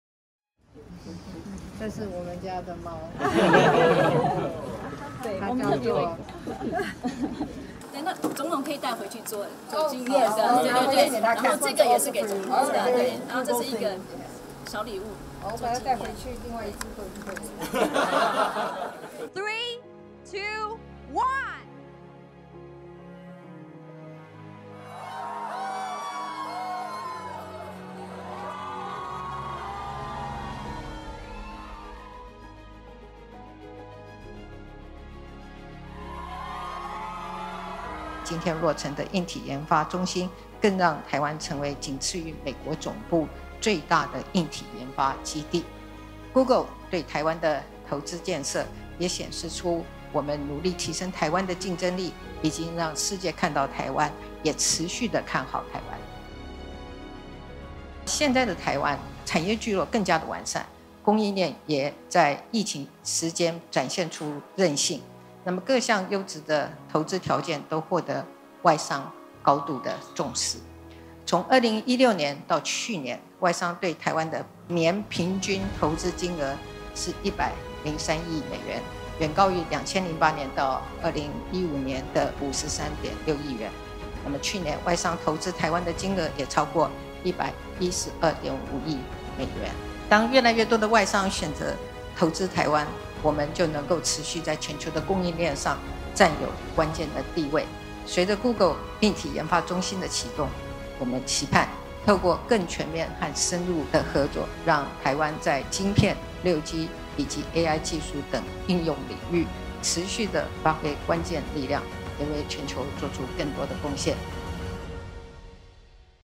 世界看見台灣 持續看好台灣｜美國境外最大硬體研發基地▶Google TPKE研發中心正式開幕！
2019年，小英總統參訪Google台灣辦公室。當時，Google也表達會持續投資台灣，要打造兆元的雲端產業。

這兩年來，Google除了在板橋設置辦公園區，4/25落成的硬體研發中心，更讓台灣成為美國境外最大的硬體研發基地。

Google對台灣的投資建設，也顯示出，我們努力提升台灣的競爭力，已經讓世界看見台灣，也持續看好台灣。

過去八年來，政府推動「5+2產業創新計畫」、「六大核心戰略產業」，以及各項基礎建設，促進台灣的進步發展。

現在的台灣，產業聚落更加完善、供應鏈也在疫情時期展現韌性，各項優質的投資條件，都獲得外商的高度重視。

從2016年到去年，外商對台灣的年平均投資金額約103億美元，遠高於2008年到2015年的53.6億美元。去年，外商投資台灣的金額，也超過112.5億美元，是近16年來的第三高。

當越來越多外商選擇投資台灣，我們也就能持續在全球供應鏈上，占有關鍵地位；台灣的國際能見度，更是不斷提升。

今天，隨著Google硬體研發中心的啟動，我們期盼，透過更全面和深入的合作，讓台灣在晶片、6G，以及AI技術應用等領域，持續發揮關鍵力量，也為全球做出更多貢獻。